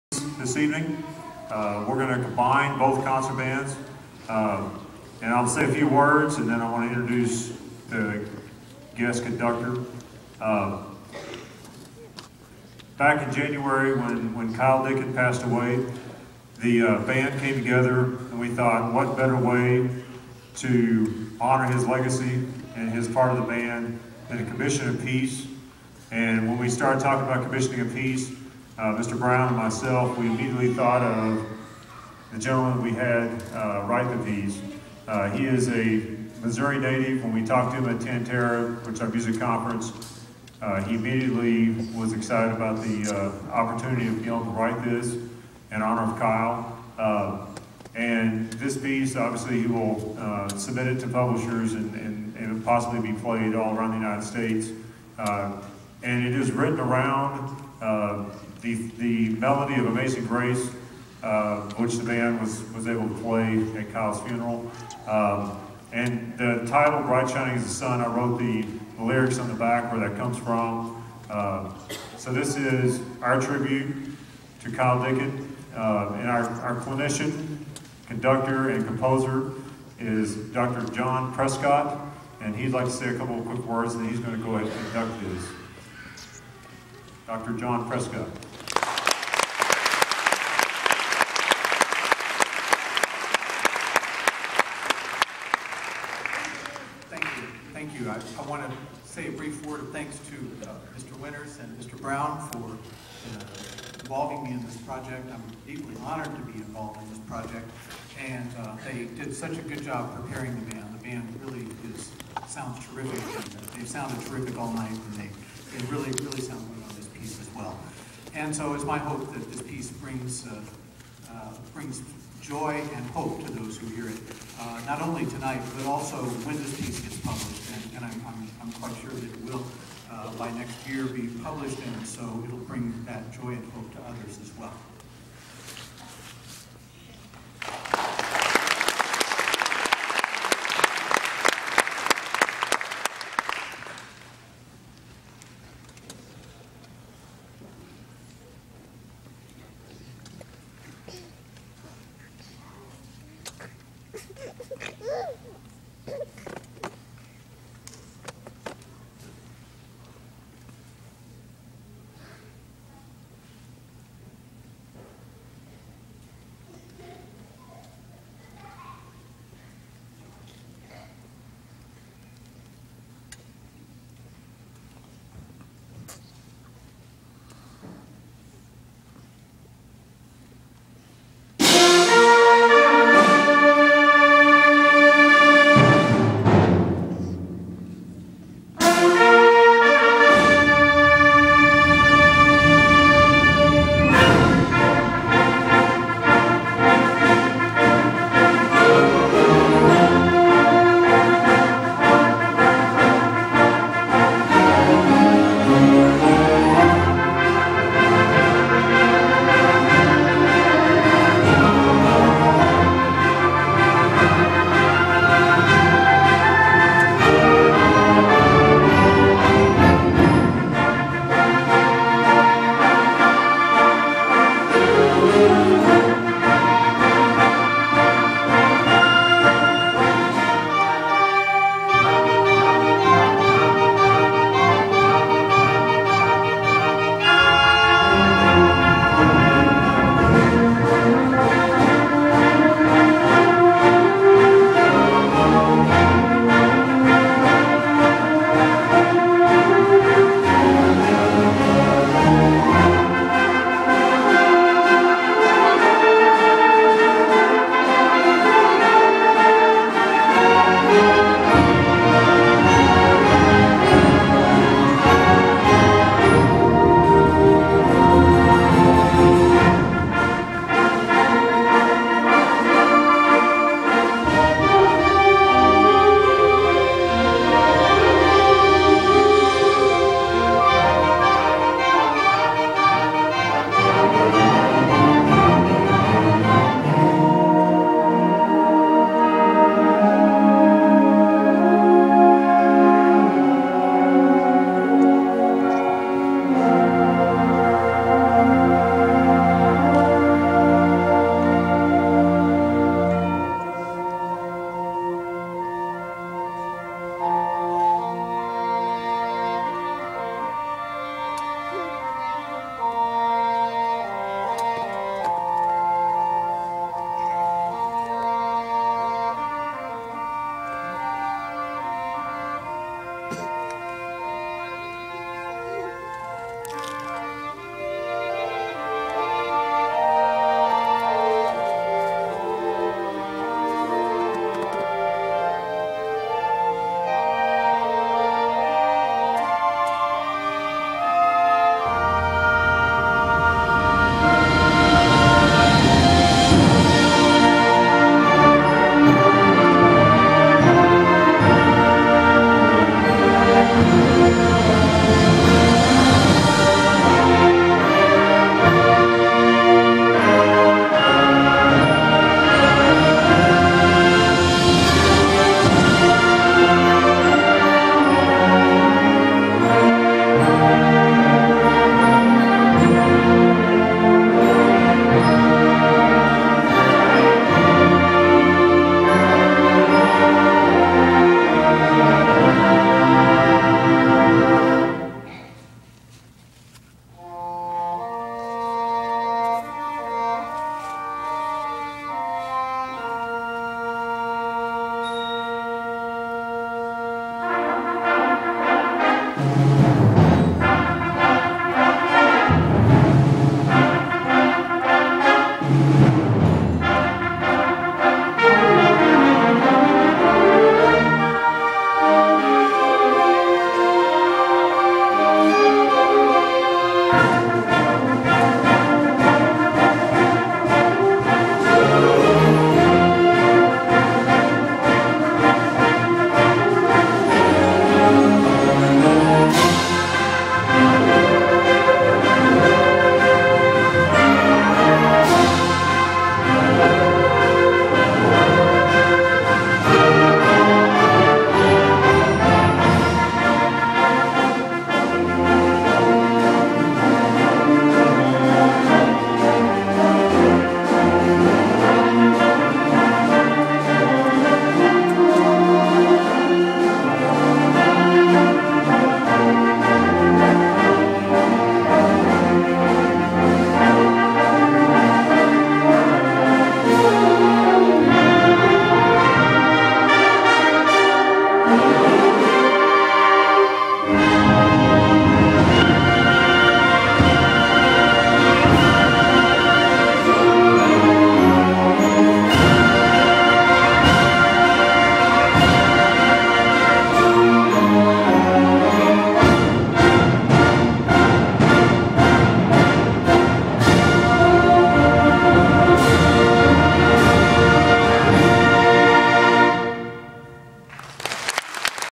Final song of the final band concert at Poplar Bluff High School. What the instructor says describes it all